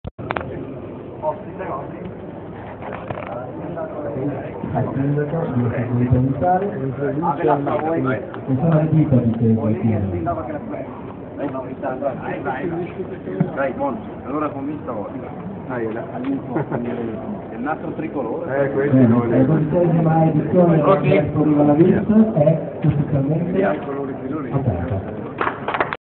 Inaugurazione Expò Palavis 24 marzo 2012
Alessandro Olivi, assessore provinciale al commercio ed il sindaco di Lavis Graziano Pellegrini inaugurano la dodicesima edizione dell'Expò Lavis.